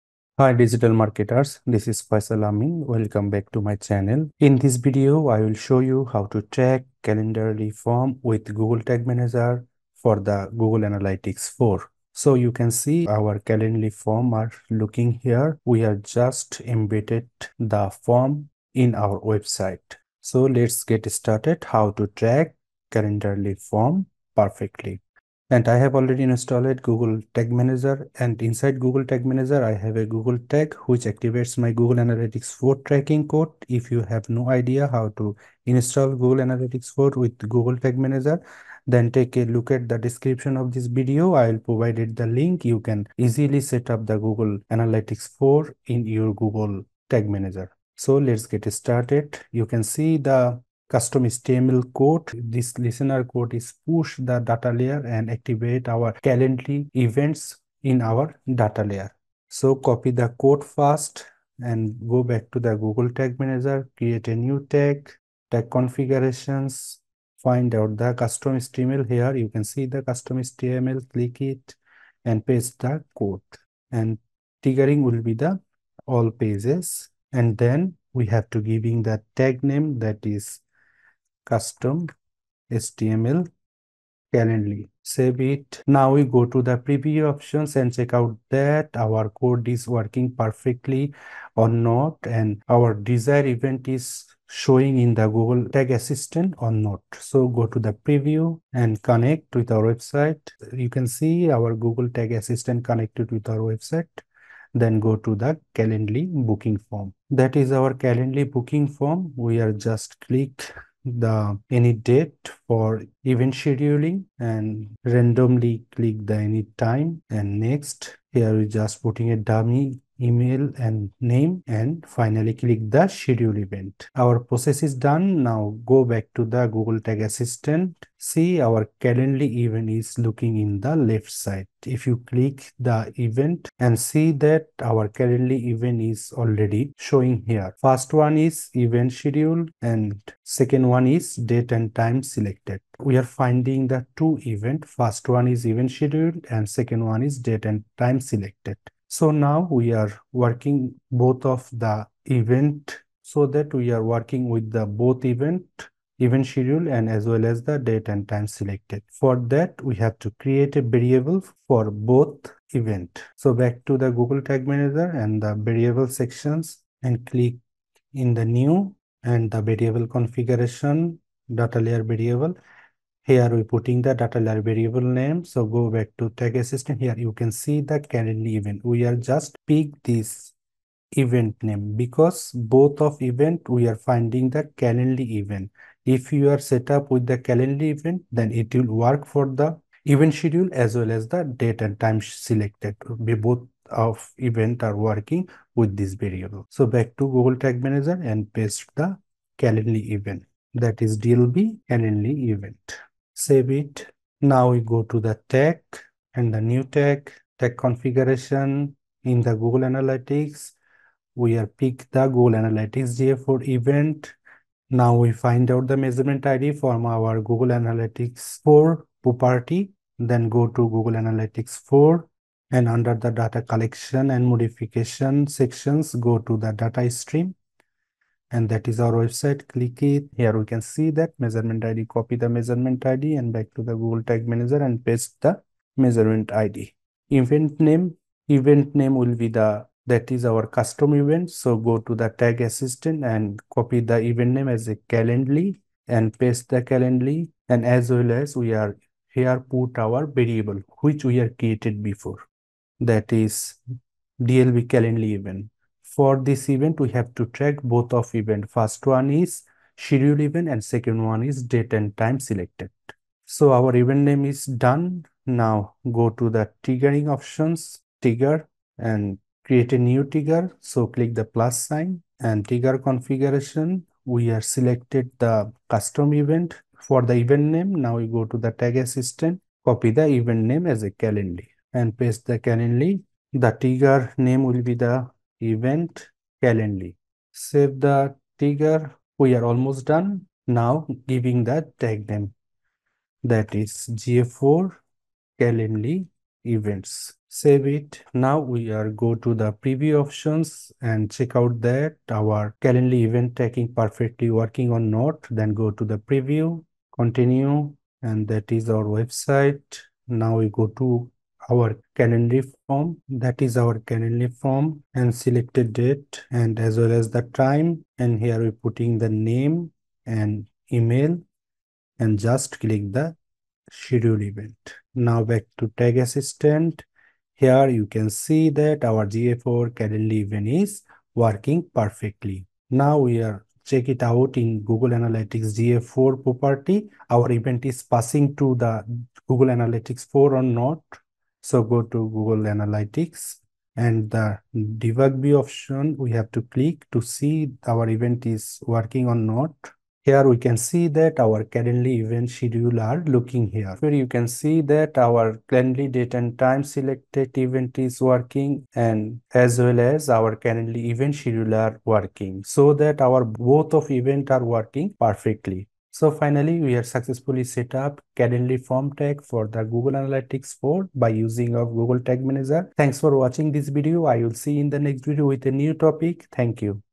How To Track Calendly Booking or Schedule Booking with GTM For GA4
Learn How to Track Calendly Form Submissions in GA4 Using GTM! In this tutorial, discover how to monitor Calendly form submissions with Google Tag Manager (GTM) and Google Analytics 4 (GA4). By activating the "event_schedule" and "date_and_time_selected" events with a listener code, and then creating a custom tag for GA4 tracking, you'll capture valuable data for analyzing user engagements and conversions through Calendly.

📅Overview of Calendly and its significance in scheduling appointments and events.
📅 Importance of tracking Calendly form submissions for understanding user interactions and optimizing scheduling processes.
📅 Implementing a listener code in GTM to activate the "event_schedule" and "date_and_time_selected" events upon user actions in Calendly.
📅 Ensuring that relevant data is pushed to the data layer for both events.
📅 Setting up a custom tag in GTM to track Calendly form submissions in GA4.
📅 Configuring the tag to fire when either the "event_schedule" or "date_and_time_selected" event is detected.
📅 Specifying event parameters to capture important data such as event type, selected date, and time.
📅 Utilizing custom event parameters to provide additional context for analyzing user interactions.
📅 Conduct thorough testing of the Calendly form tracking implementation to ensure accurate data collection.
📅 Using GTM preview mode and GA4 real-time reports to validate form submission tracking.

✨ MD FAISAL AMIN — Tracking & Analytics Specialist
GA4 • GTM • Meta CAPI • Server-Side Tracking Expert